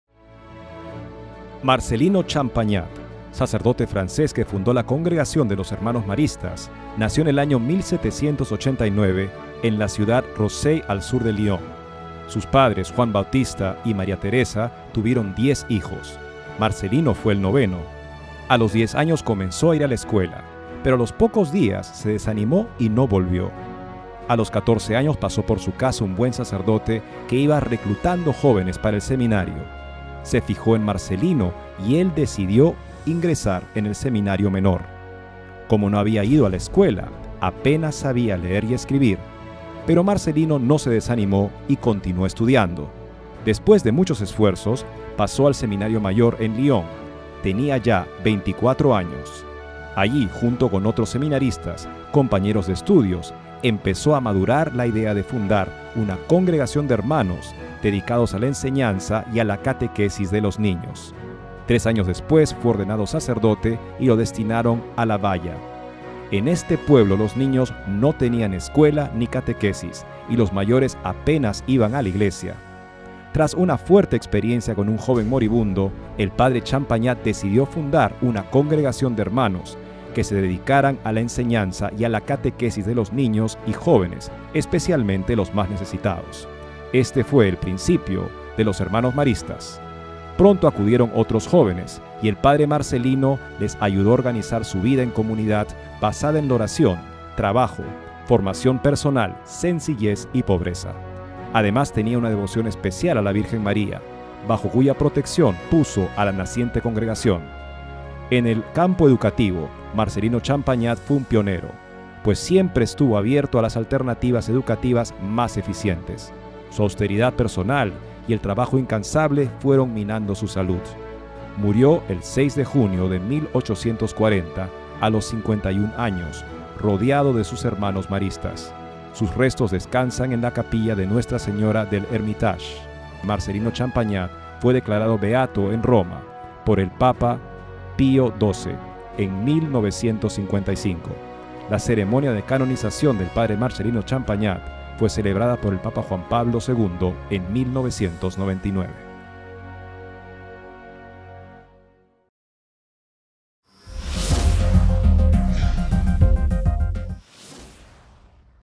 San Marcelino Champagnat
Marcelino Champagnat, sacerdote francés que fundó la congregación de los Hermanos Maristas. Nació el año 1789, el mismo año de la Revolución Francesa, en Rosey al sur de Lyon. Sus padres, Juan Bautista y María Teresa, tuvieron 10 hijos, Marcelino fue el noveno. Durante su infancia, trabajó en casa: su familia poseía una pequeña granja y un molino. A los diez años comenzó a ir a la escuela, pero a los pocos días se desanimó y no volvió. A los catorce años, pasó por su casa un buen sacerdote que iba "reclutando" jóvenes para el seminario; se fijó en Marcelino y le animó: "Tienes que estudiar para ser sacerdote. Dios lo quiere." Y Marcelino se decidió.